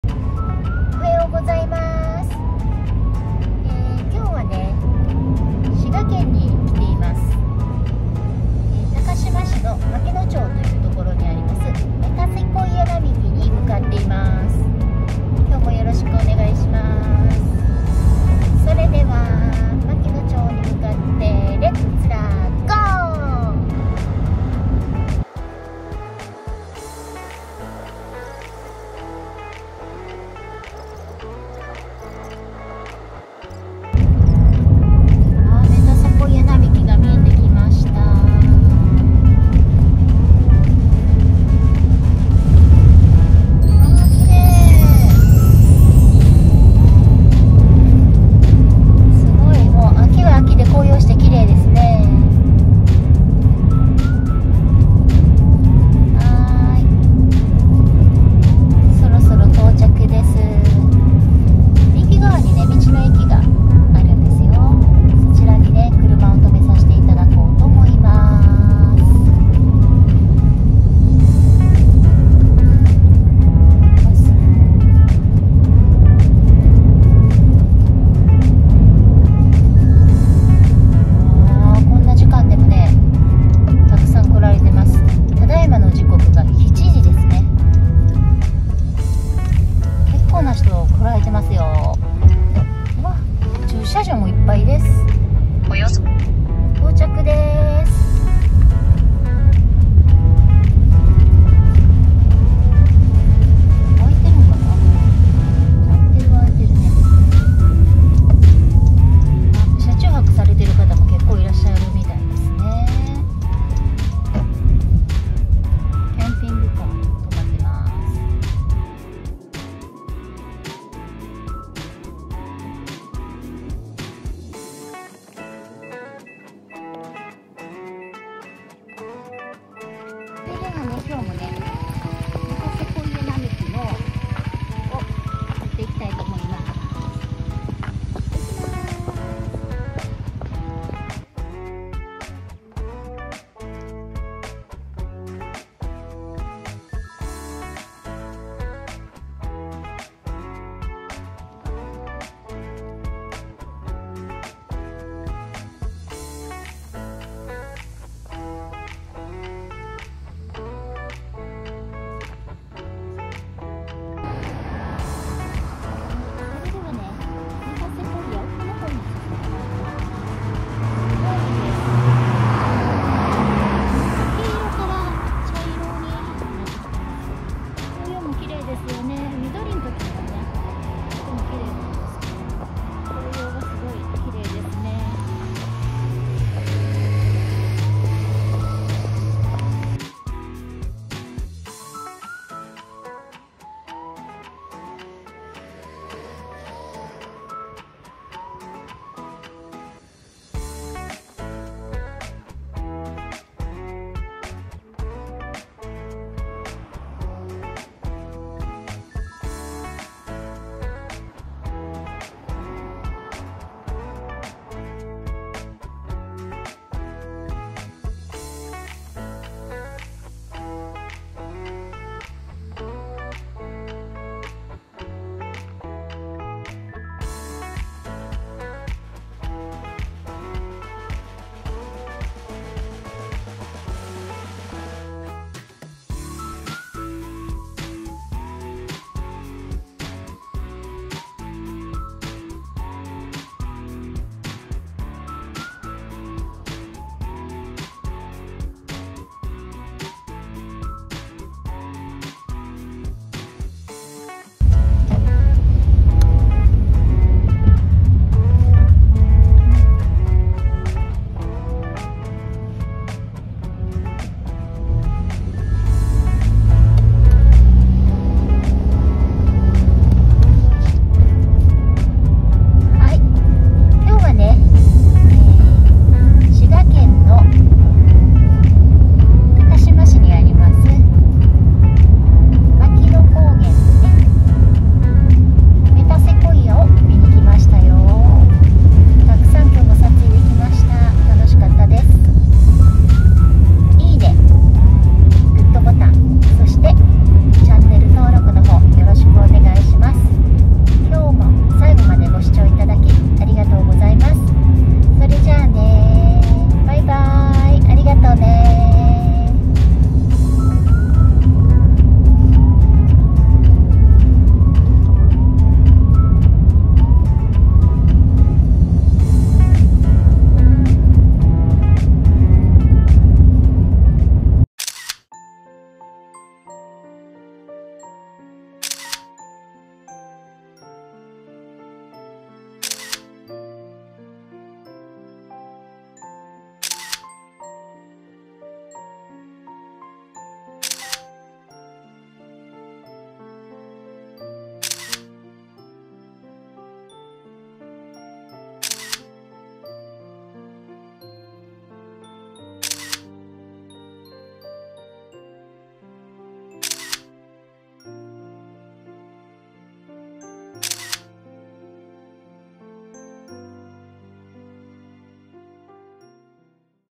滋賀県高島市マキノ町のメタセコイア並木へ行ってきました🚙メタセコイア並木 滋賀県 紅葉スポット 写真撮ってる人と繋がりたい い
最後に撮影した写真を載せているので見てくださいねっ！